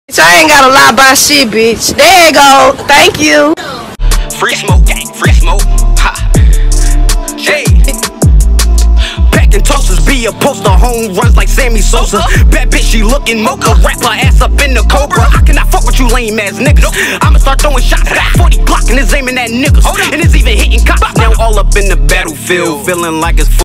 Nbayoungboy NBA Youngboy Girlfriend is definitely pregnant
Jania showing off her baby bump🙏🏽